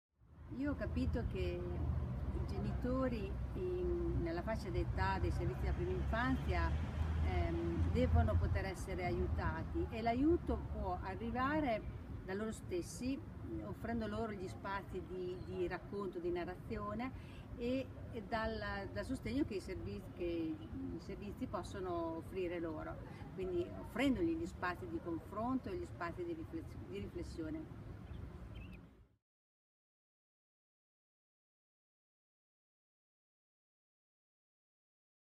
Becoming Familiar (Extract)
Paola, from a small town in the mountains of Modena, Italy works for the local administration. Through the years she has gained awareness of the importance of offering to the families of young children some spaces where they can meet and share their experiences in educating their children in order to support each other.  In this clip she highlights the importance of storytelling and reflection.